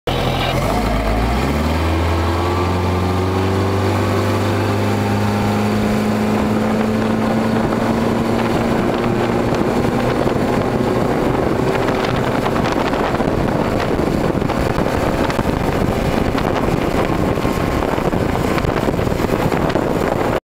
Hydrodyne test run after sitting 27 years.
1962 hydrodyne